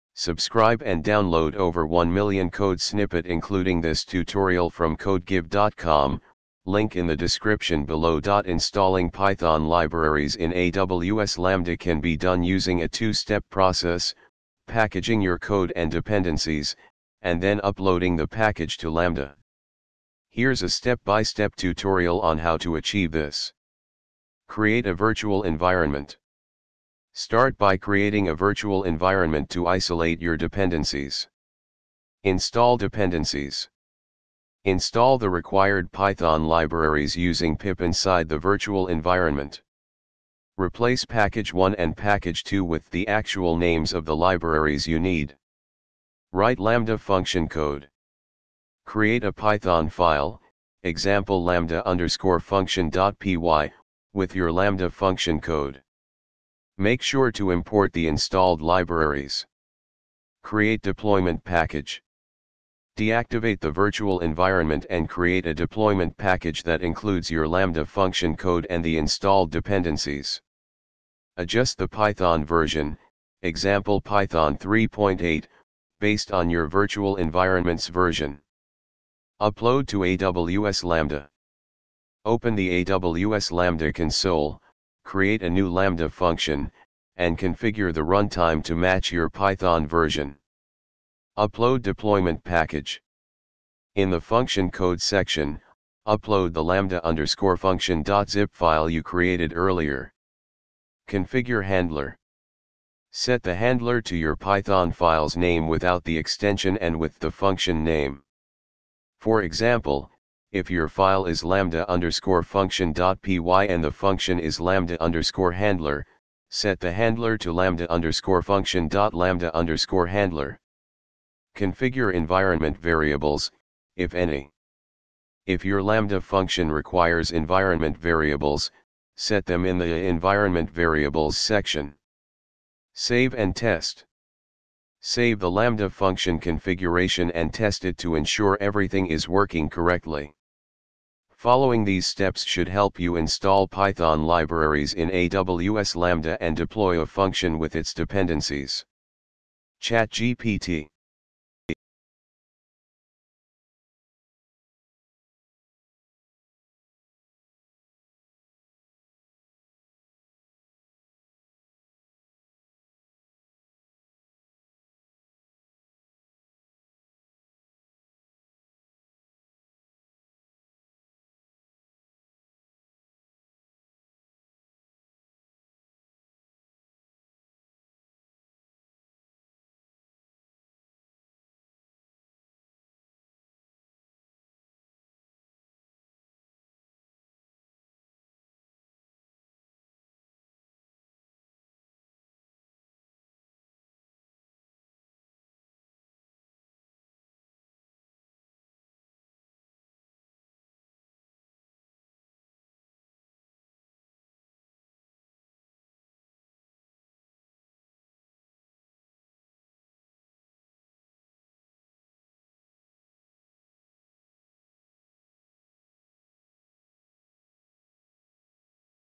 how to install python libraries in aws lambda
Installing Python libraries in AWS Lambda can be done using a two-step process: packaging your code and dependencies, and then uploading the package to Lambda. Here's a step-by-step tutorial on how to achieve this:
Create a Virtual Environment:
Start by creating a virtual environment to isolate your dependencies.
Install Dependencies:
Install the required Python libraries using pip inside the virtual environment.
Replace package1 and package2 with the actual names of the libraries you need.
Write Lambda Function Code:
Create a Python file (e.g., lambda_function.py) with your Lambda function code. Make sure to import the installed libraries.
Create Deployment Package:
Deactivate the virtual environment and create a deployment package that includes your Lambda function code and the installed dependencies.
Adjust the Python version (e.g., python3.8) based on your virtual environment's version.
Upload to AWS Lambda:
Open the AWS Lambda console, create a new Lambda function, and configure the runtime to match your Python version.
Upload Deployment Package:
In the "Function code" section, upload the lambda_function.zip file you created earlier.
Configure Handler:
Set the "Handler" to your Python file's name without the extension and with the function name. For example, if your file is lambda_function.py and the function is lambda_handler, set the handler to lambda_function.lambda_handler.
Configure Environment Variables (if any):
If your Lambda function requires environment variables, set them in the "Environment variables" section.
Save and Test:
Save the Lambda function configuration and test it to ensure everything is working correctly.
Following these steps should help you install Python libraries in AWS Lambda and deploy a function with its dependencies.
ChatGPT